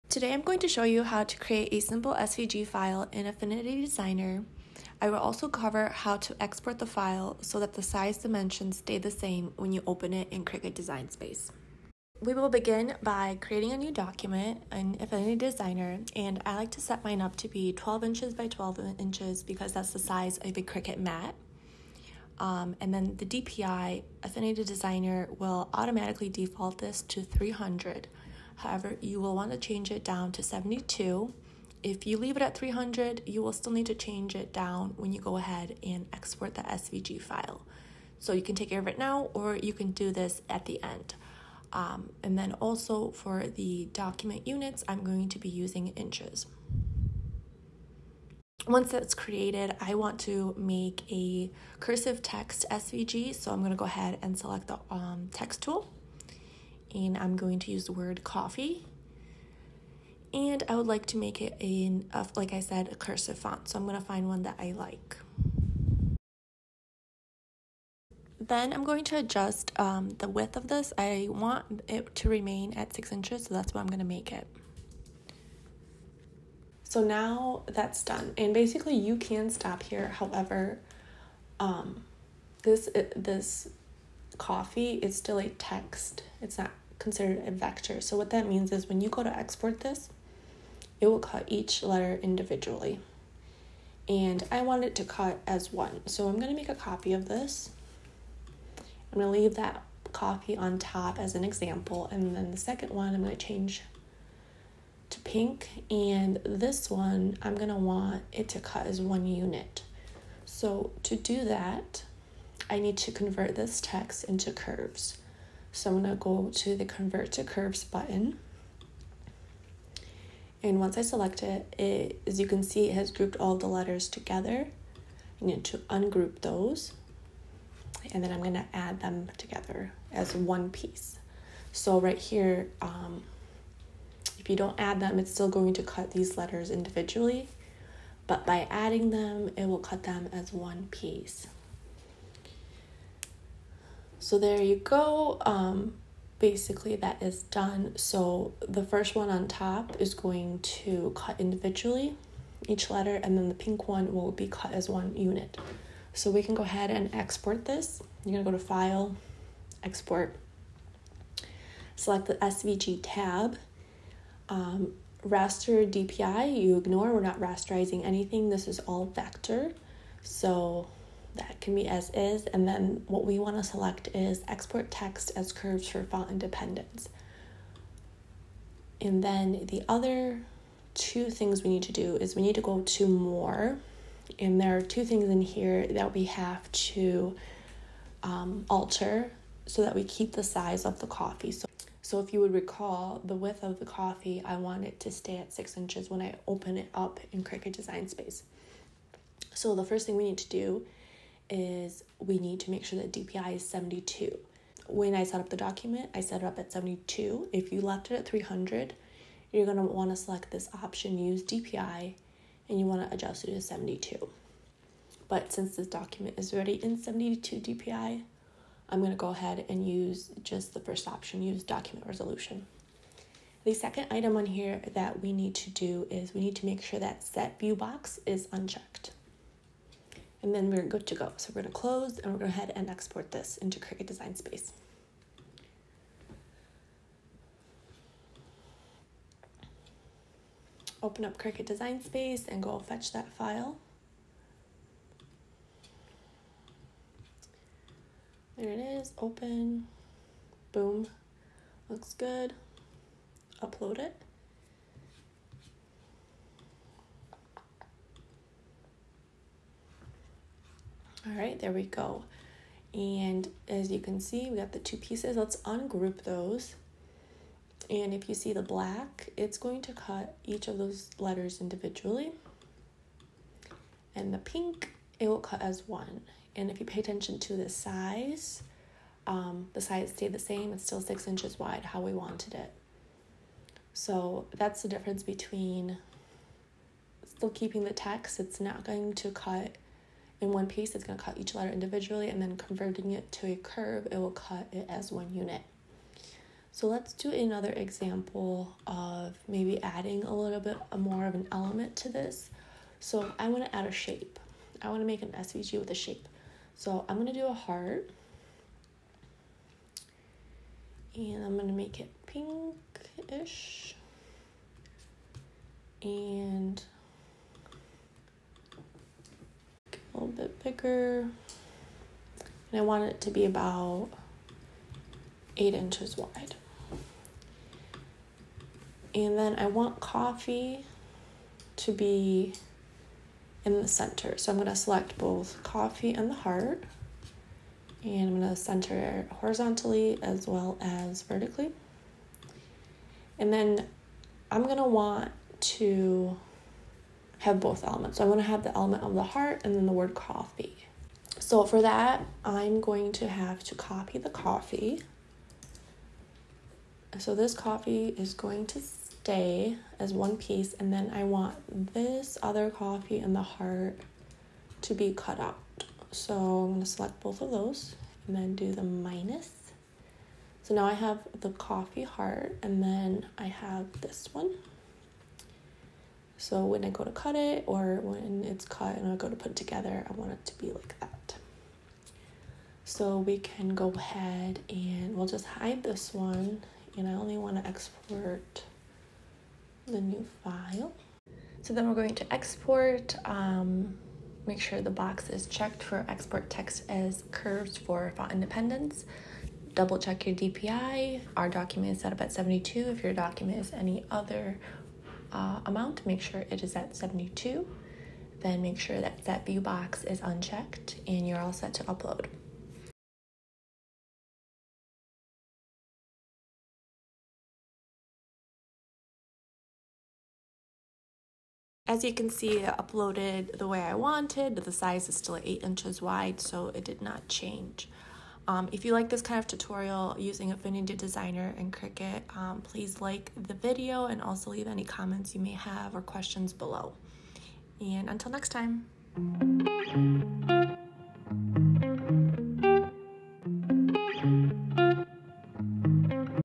How to Create an SVG in Affinity Designer Then Export to Cricut Design Space without Size Changing
In this video I go over how to create an SVG file in Affinity Designer then export that file into Cricut Design space without the size changing.